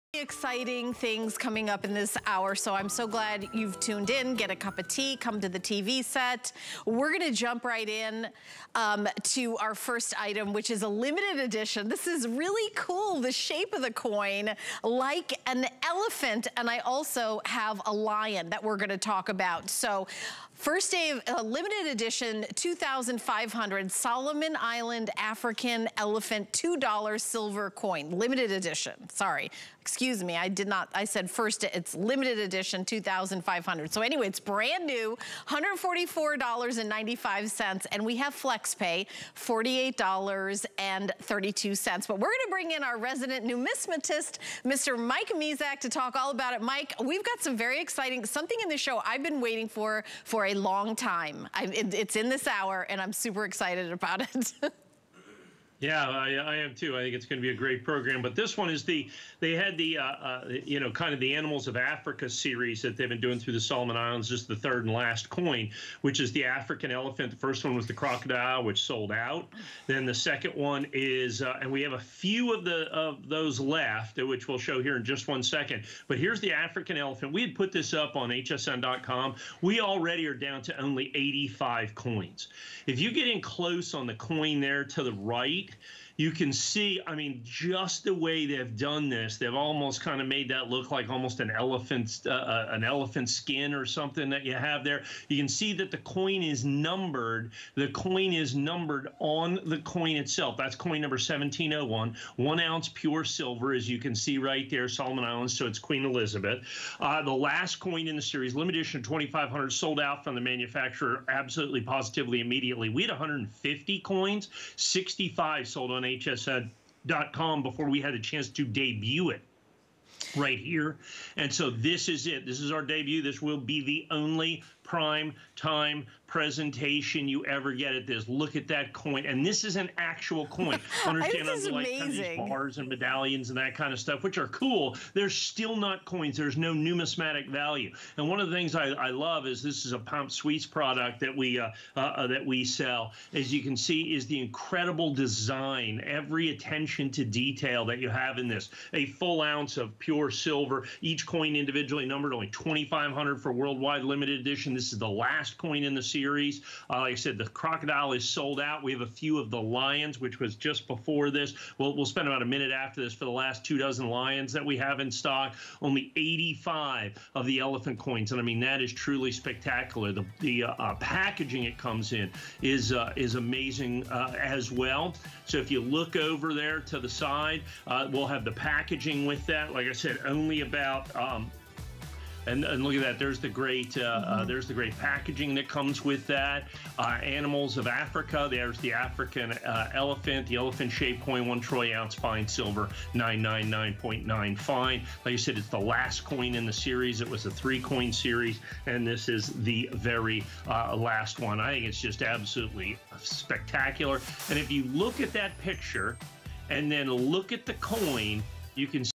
2021 LE 2,500 Animals of Africa Series African Elephant ...
2021 LE 2,500 Animals of Africa Series African Elephant $2 Silver Coin
Celebrate the African Elephant  the largest land animal on Earth  and a keystone species, making its role in the ecosystem critical. It's also
the third coin in the Animals of Africa series. The first two (Crocodile and Lion) are sold out, and there's only 2,500 to go around. Nab yours now.

    African elephant silver $2 Solomon Islands coin  limited edition of 2,500
    Original African Elephant inspired box